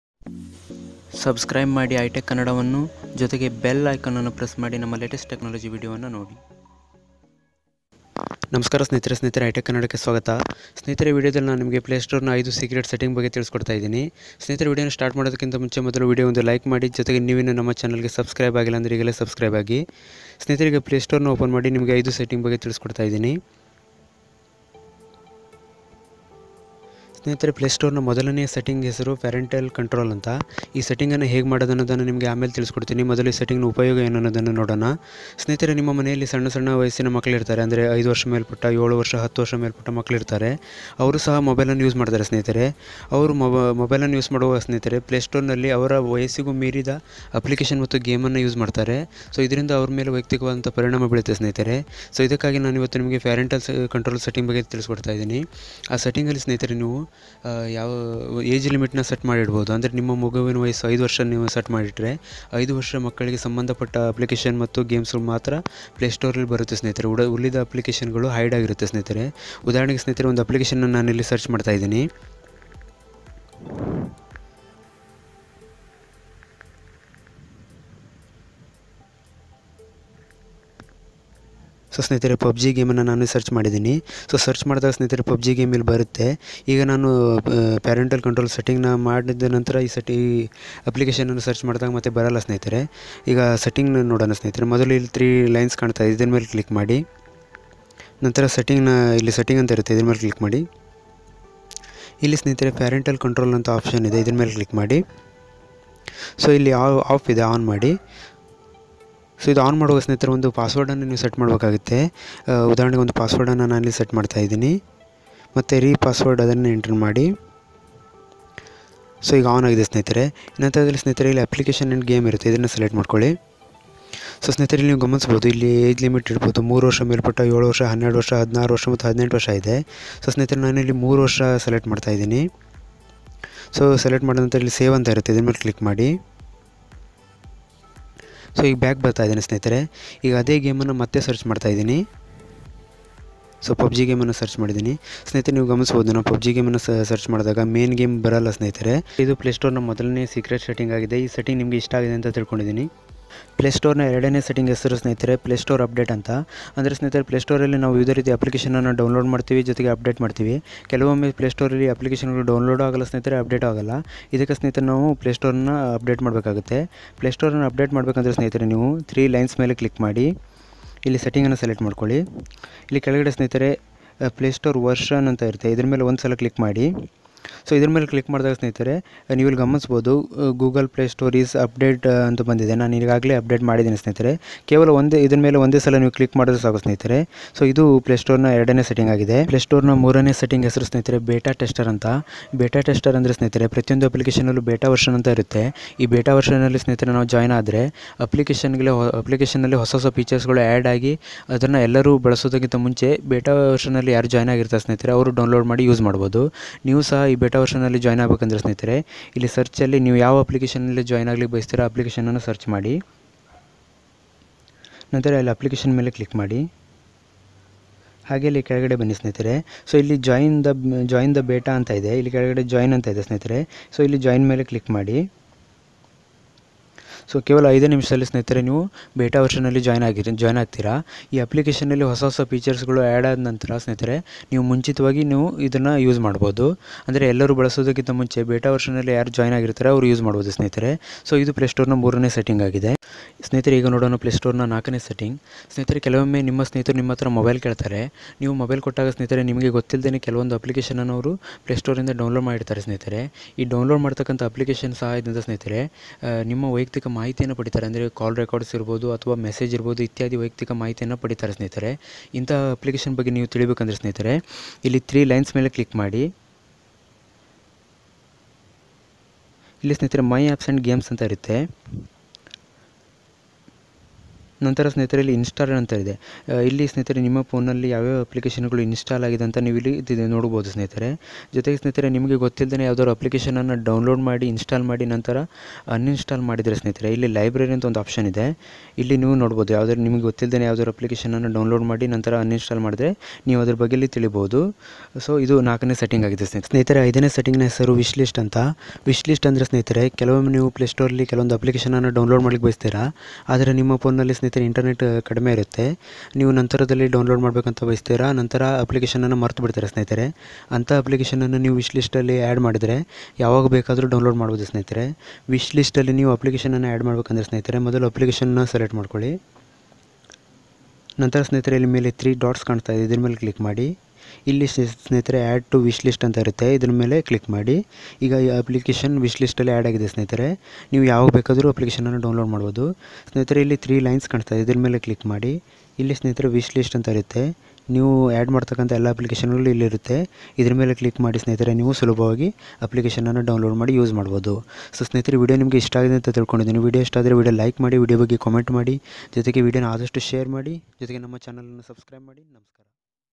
Play Store 5 Secret Settings ||by i tech kannada
Hello friends

In this video I will tell you top 5 most useful Play store settings for all smart phone users

Play store secret settings
Play store updates
How to join beta version
Play store whishlist
Play store hidden features

▶️Comments

I TECH KANNADA